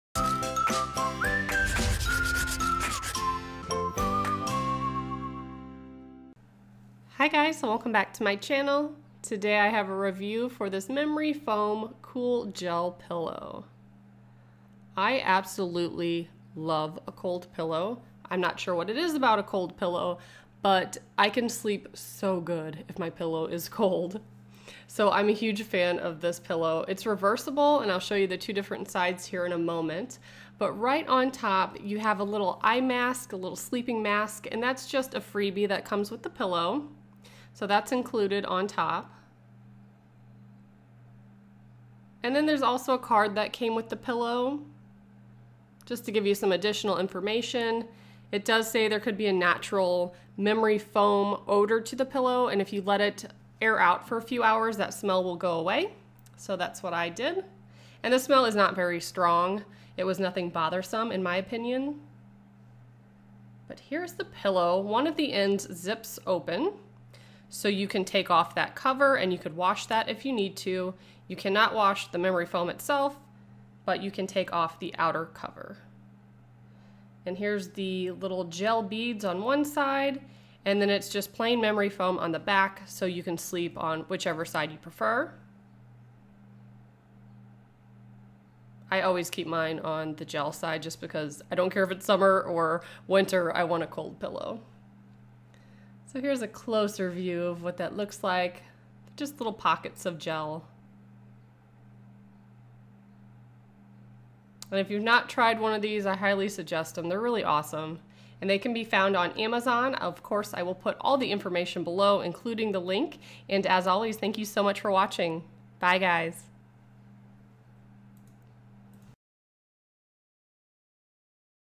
Reversible Gel Memory Foam Pillow - Review!
Cr Comfort & Relax Reversible Memory Foam Gel Pillow for Sleeping Cool

• NECK SUPPORT - It conforms to the curve of your head and neck to give enough support so that you can sleep well

• COOL GEL PAD - Help disperse the heat so that you can sleep cool longer

• Two Choice for Sleep on - One side is cool gel pad for sleeping cool and the other side is memory foam pad, ventilated

• CertiPUR-US certified memory foam means does not contain any toxic ingredients like formaldehyde, methane chloride, or PBDE

• OUTER COVER can be removable and washable. NOTICE: Do not put inner memory foam fill in washer or dryer

What’s on my nails? Flora Good Time